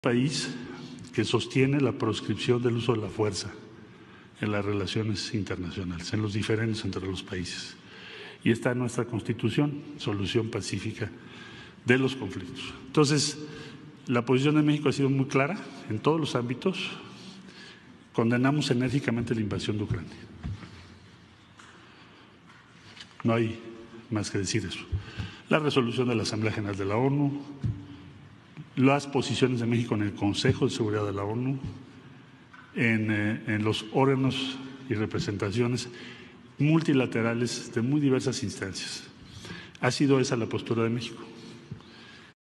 México promueve la ayuda humanitaria a Ucrania y no se van imponer sanciones a Rusia
“Condenamos enérgicamente la invasión de Ucrania", reiteró el canciller Marcelo Ebrard durante la mañanera. Además, señaló que México promueve la ayuda humanitaria y negó que se vayan a imponer sanciones a Rusia.